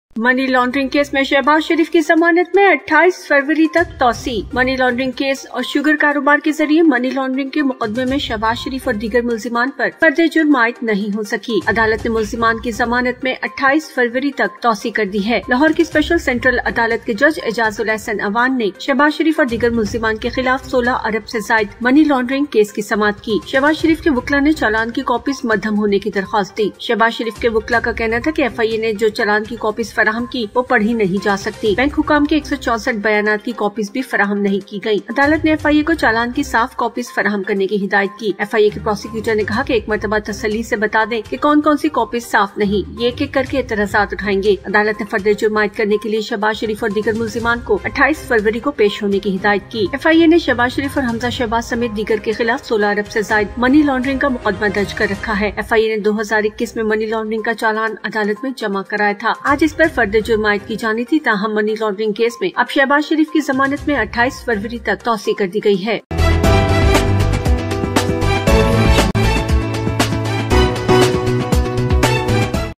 Shahbaz Sharif could not be indicted in money laundering case
Shahbaz Sharif's lawyers request copies of challan to be dimmed. Shahbaz Sharif's lawyers said that the copies of the challan provided by the FIA ​​could not be read. Bank officials also did not provide copies of the 164 statements. The court directed the FIA ​​to provide clear copies of the challan